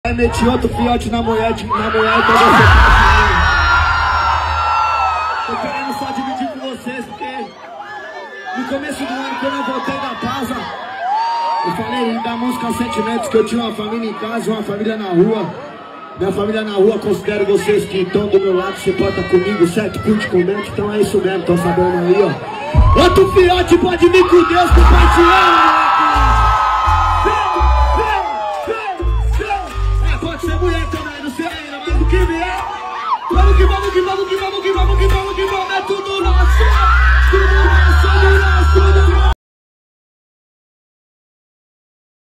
MC HARIEL VAI SER PAI DE NOVO😍 ANUNCIA QUE ESTA ESPERANDO SEU 2 FILHO EM SEU SHOW
Em um Show em Floripa MC HARIEL acabou de anunciar que está esperando seu segundo filho ou filha. Veja o momento